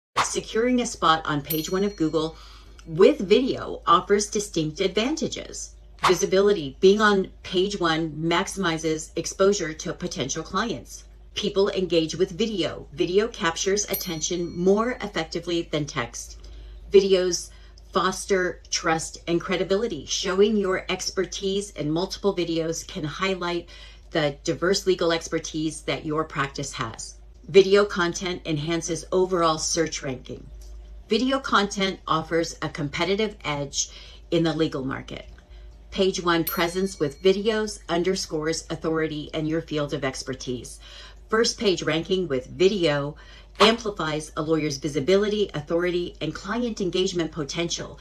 Why Google's Top Spot is Essential!🌐
🌐Beyond the First Page: Why Google's Top Spot is Essential!🌐
Description: "Discover why being on the first page of Google is non-negotiable for online success. From trust-building to organic traffic, we'll show you the way! 🌟📊 GoogleSEO DigitalMarketingTips FirstPageAdvantage"